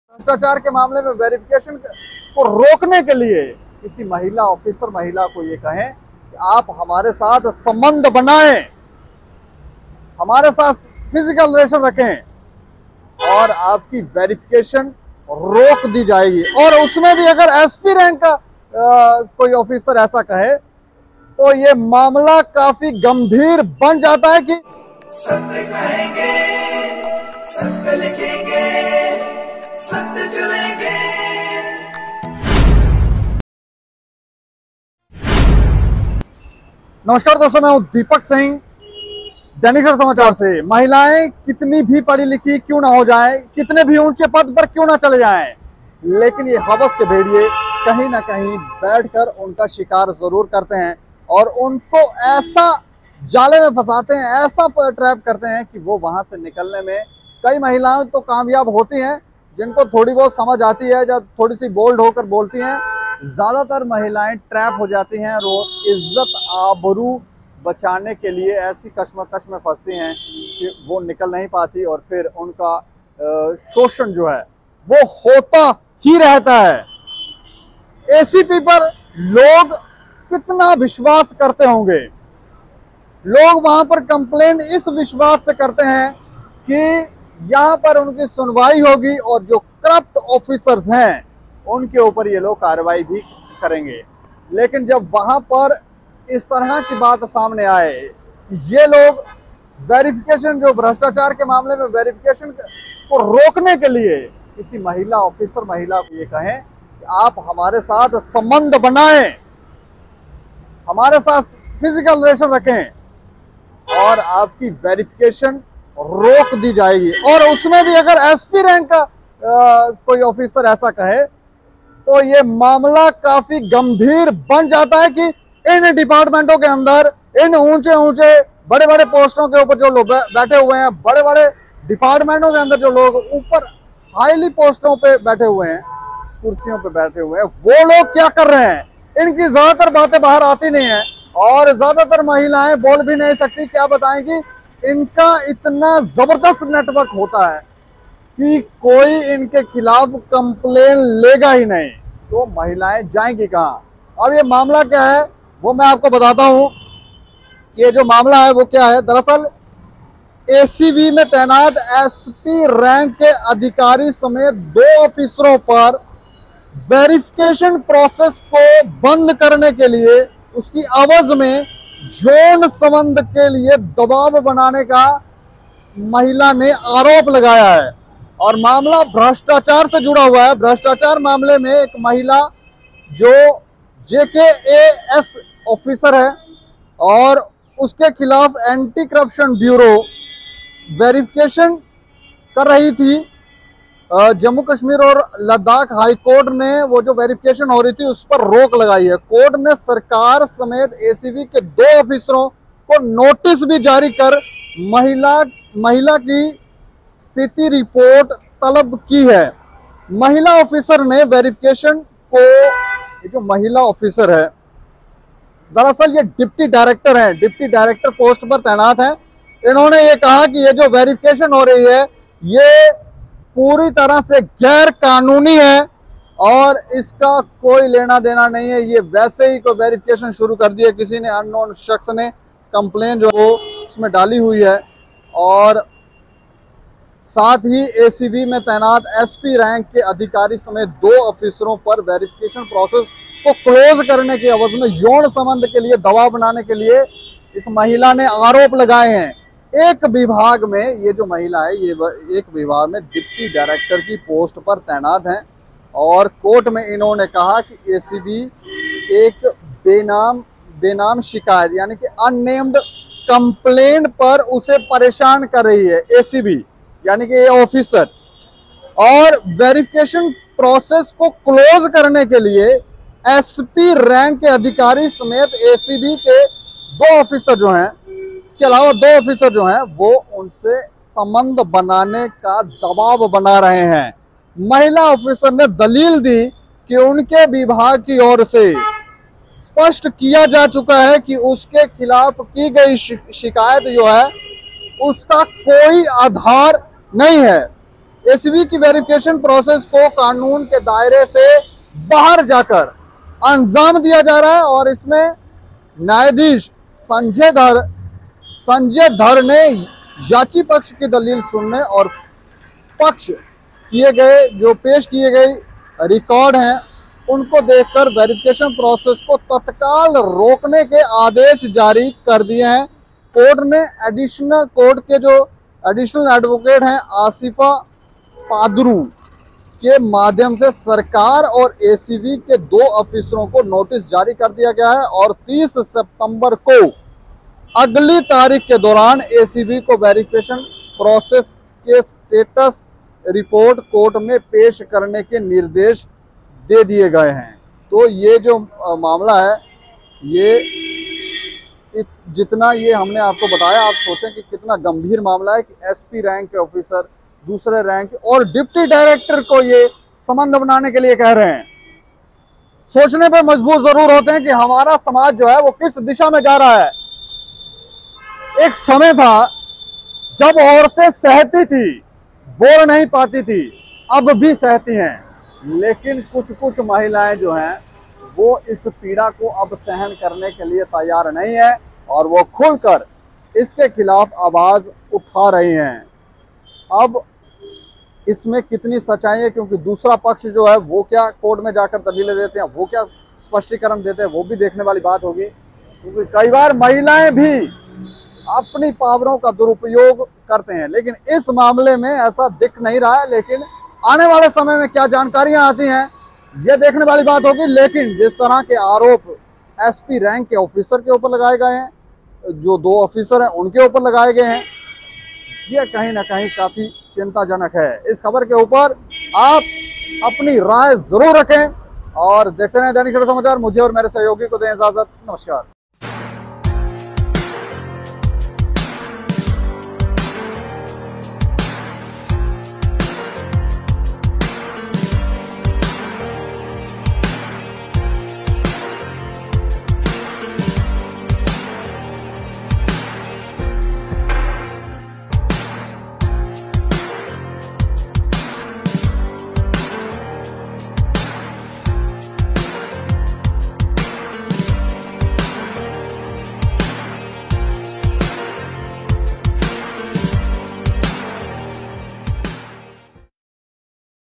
एक महिला Officer ने SP Rank के अधिकारी पर Sexual Harassment का केस दर्ज करवाया || State Samachar
SexualHarassment: Deputy Director की Post पर तैनात महिला Officer ने ACB में तैनात SP Rank के अधिकारी पर Sexual Harassment का केस दर्ज करवाया !!